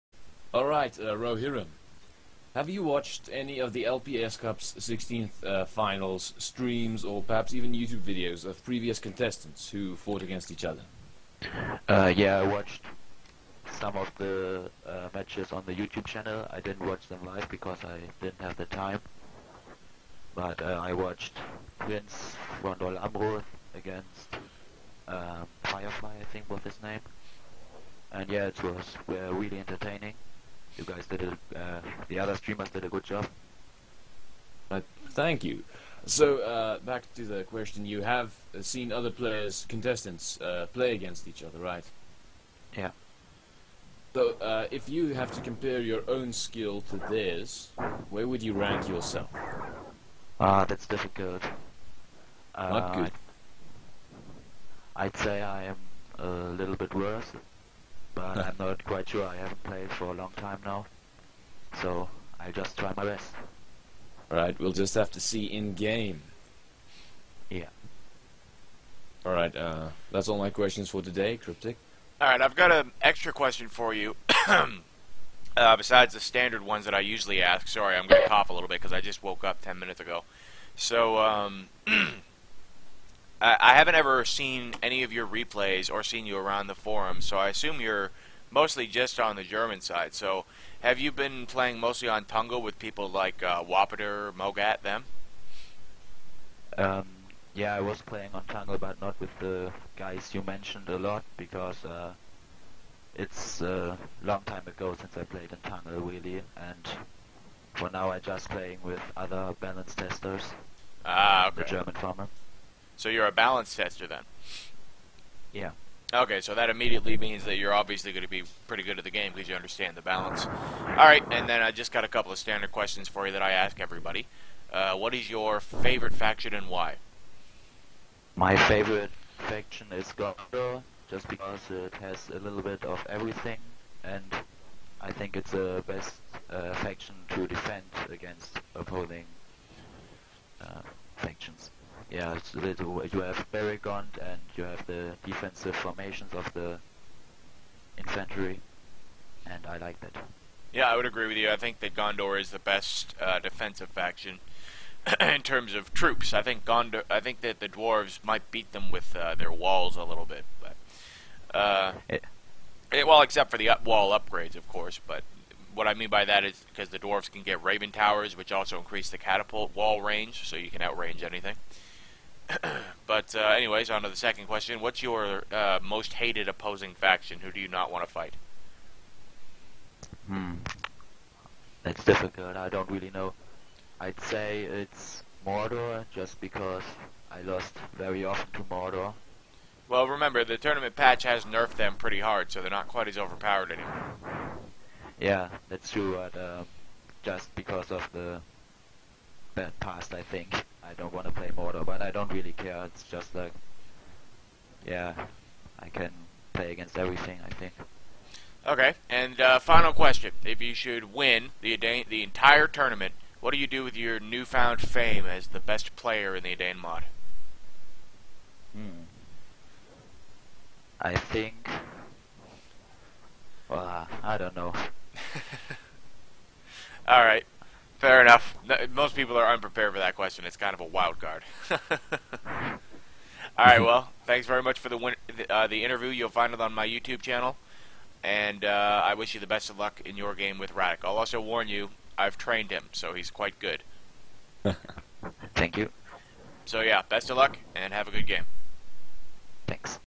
LPS Cup 16th Finals Rohirrim Interview
Exclusive interview with Rohirrim before his games in the 16th finals!